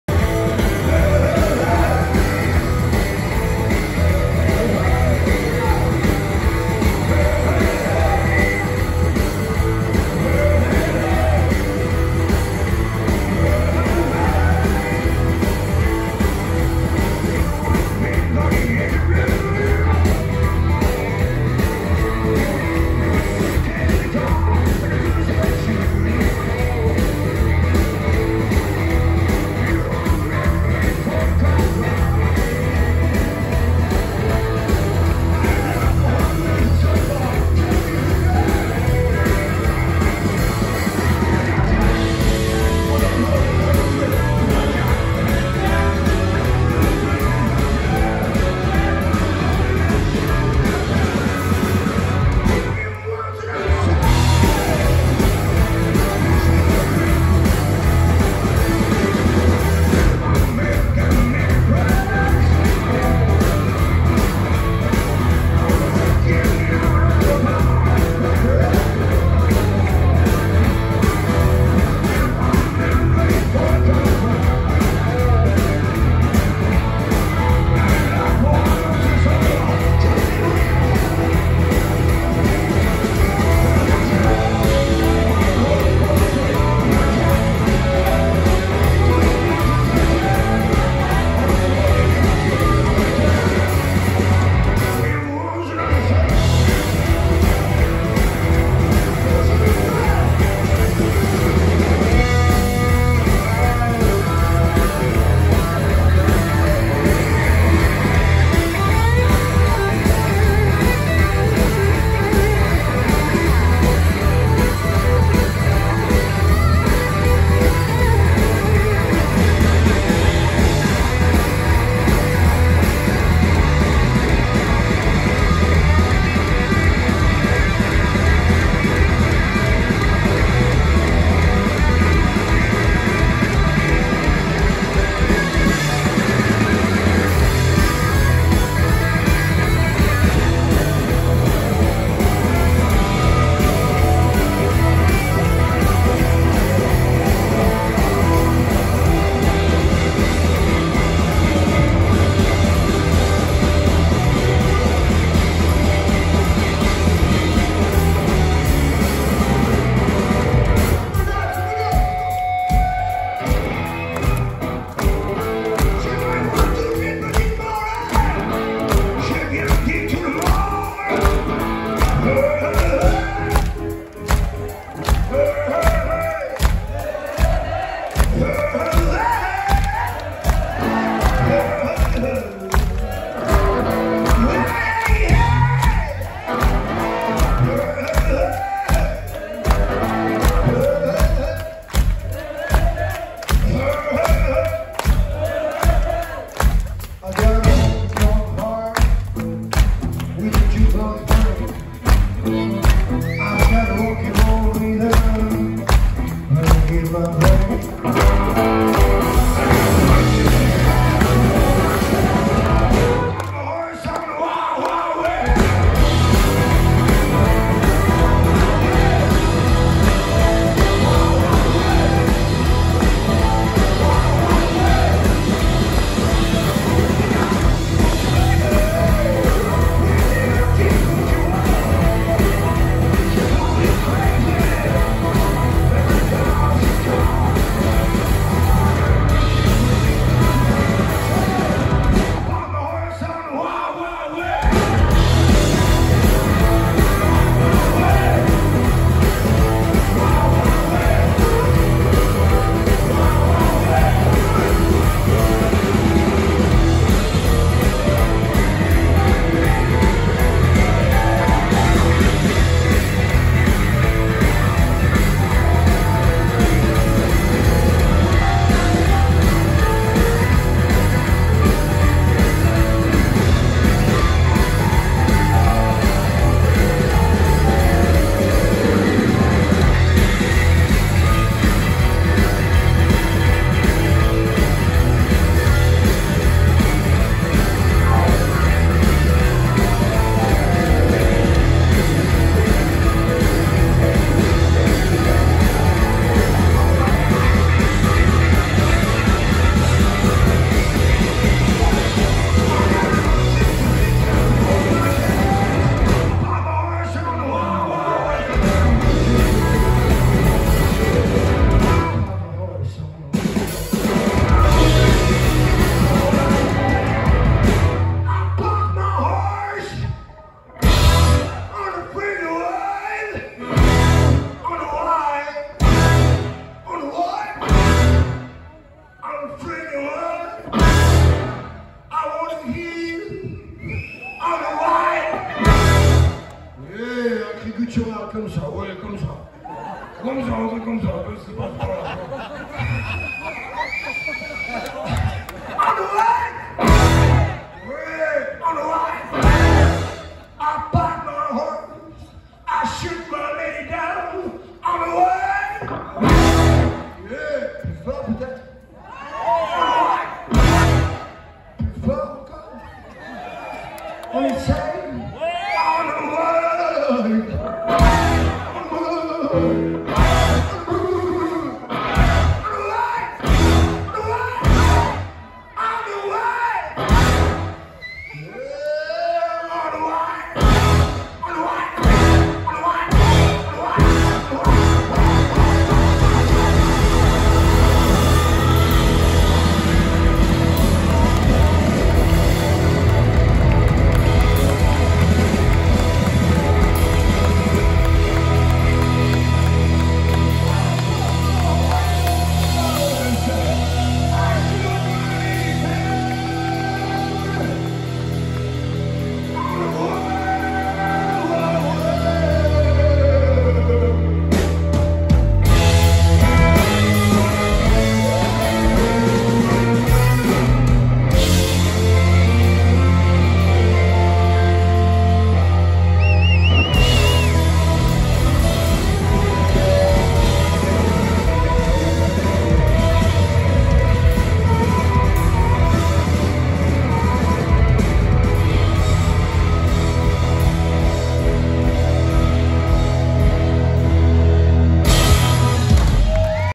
Manu Lanvin, Dax 2022 (4)
Manu Lanvin en concert sur la grande scène du Dax Motors'n Blues Festival le 15 juillet 2022.